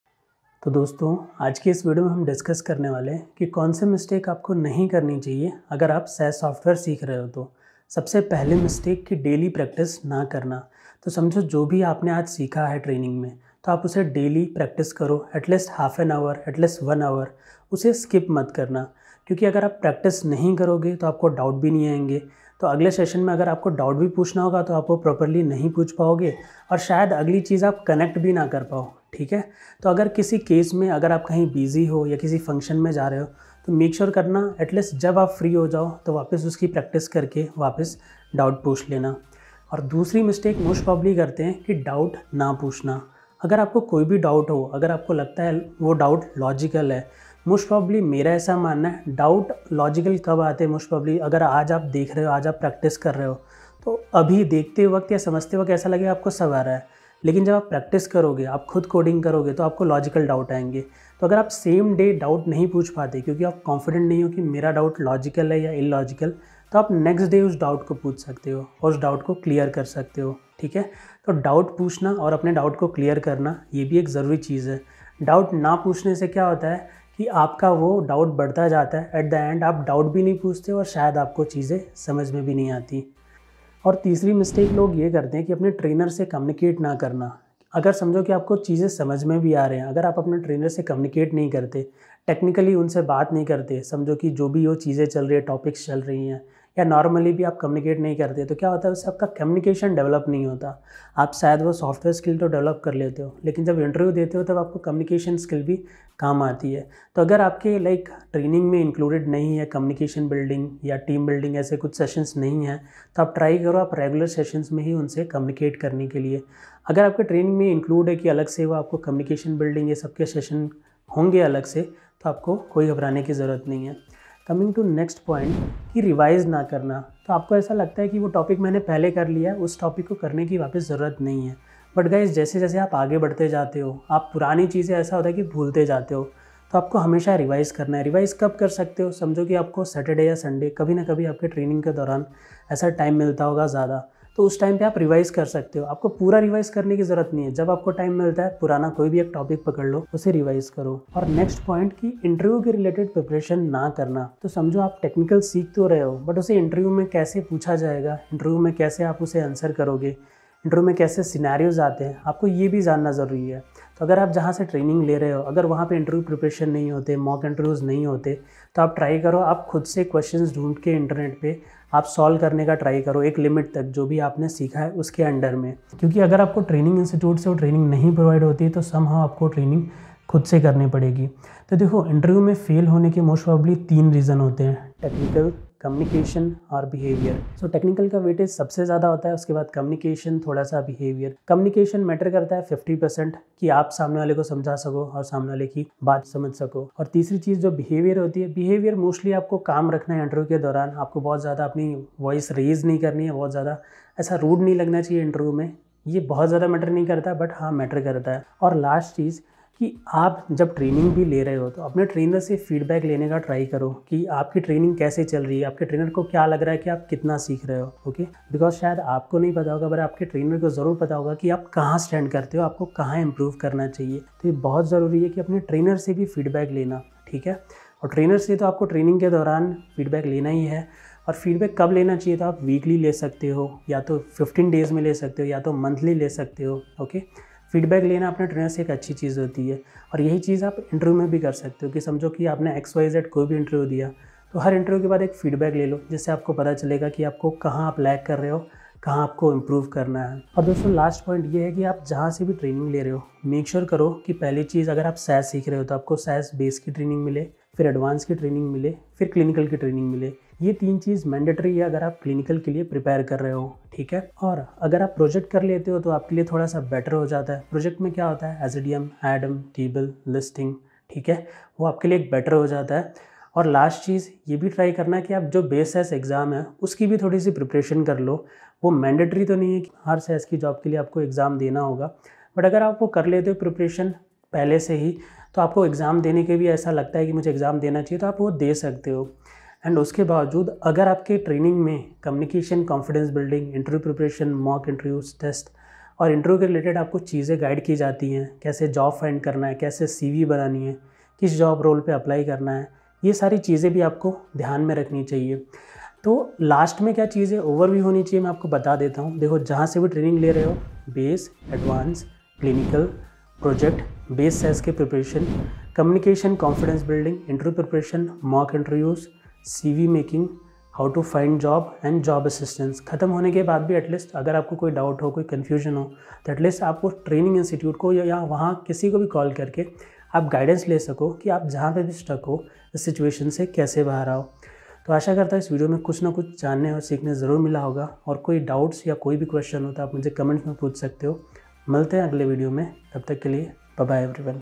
Avoid these Mistakes while learning SAS or any other language | For students | Mr.Seeker
Do you want to master SAS in no time? Then don't miss this video! I'm showing you the common mistakes that beginner SAS learners make and how to prevent them. With the tips I'm sharing here, you'll be able to learn SAS faster & more efficiently and get a jumpstart on your career. Watch now and start taking advantage of these helpful tips!

Interested in learning SAS? Don't make the same mistakes that thousands of other SAS learners have made! In this video, I'll show you some of the common mistakes that beginners make while learning SAS and how to avoid them. Learn from my experience and save time & energy with this quick & easy guide.